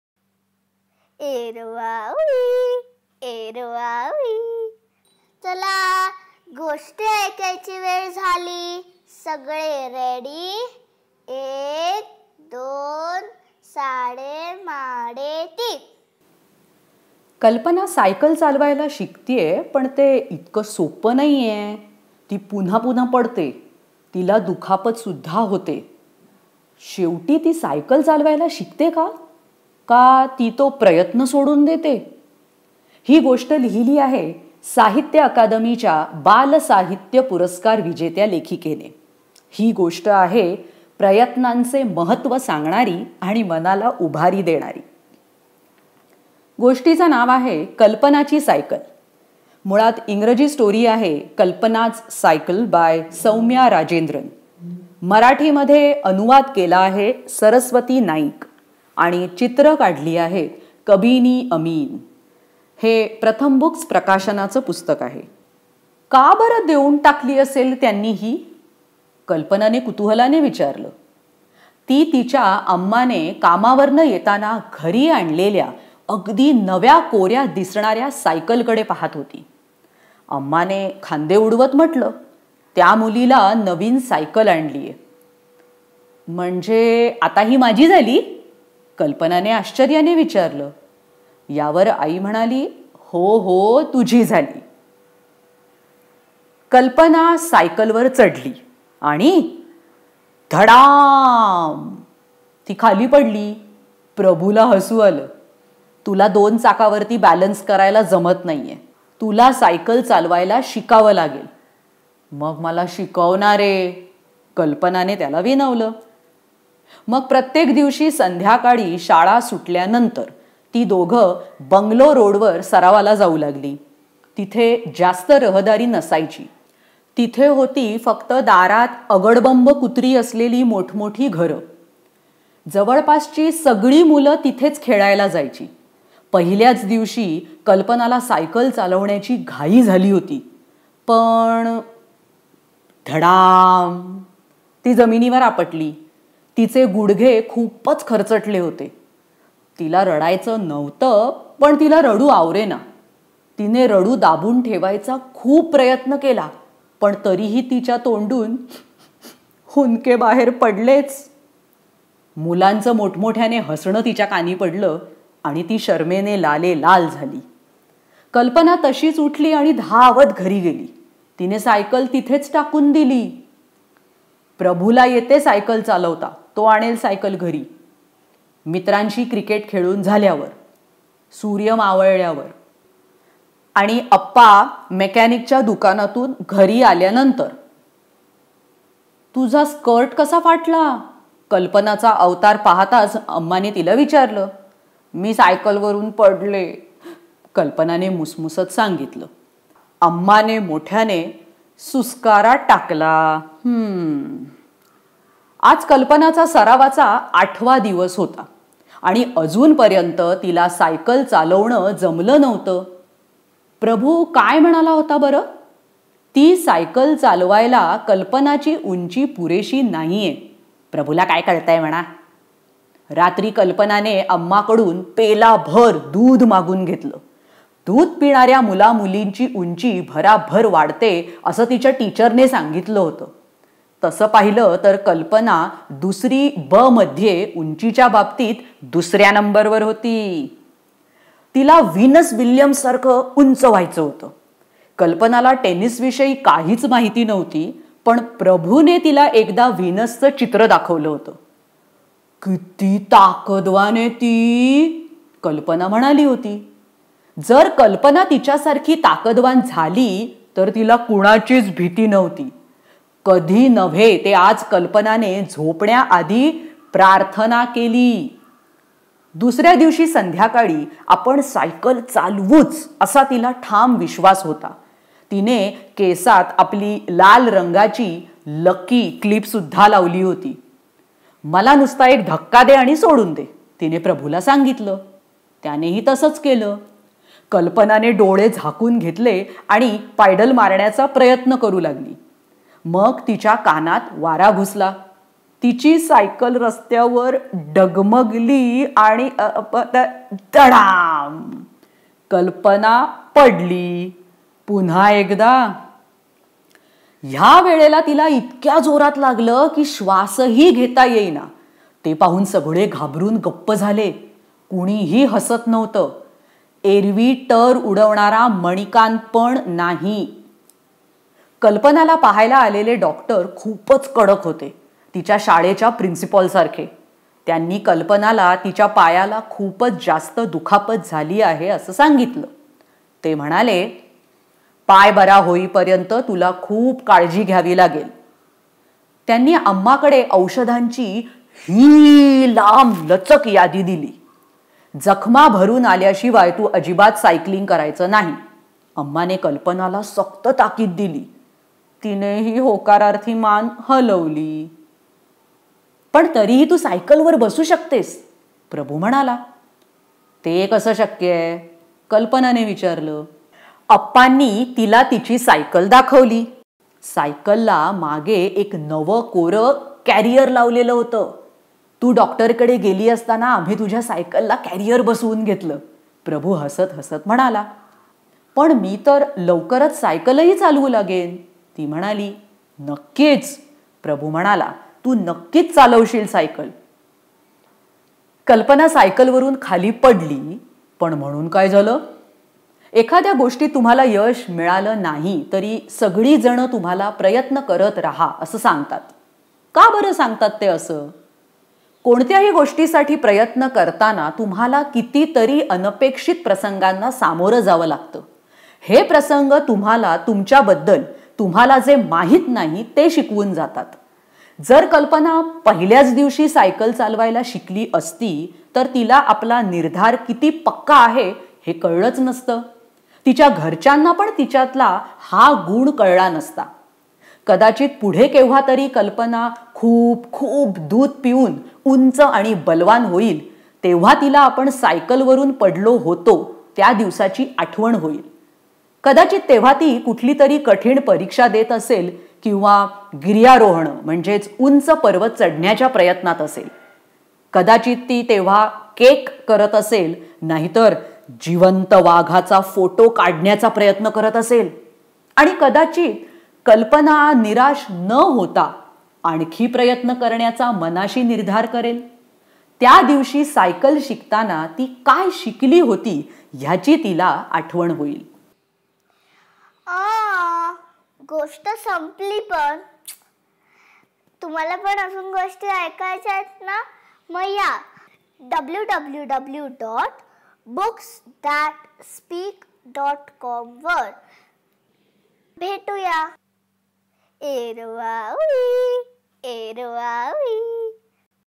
Marathi Stories for Kids - Kalpanachi cycle ( Kalpana's Cycle ) - Pratham Books
Kalpana is learning to cycle, but it isn’t easy at all! She falls over and over again, even hurting herself. Does she finally learn to cycle? Does she give up? A heartwarming story about the importance of trying.

Original story Kalpana's Cycle by Sowmya Rajendran, Translated into Marathi by Saraswati Naik, Illustrated by Kabini Amin.
Published by Pratham Books. Narrated by Asawari Doshi.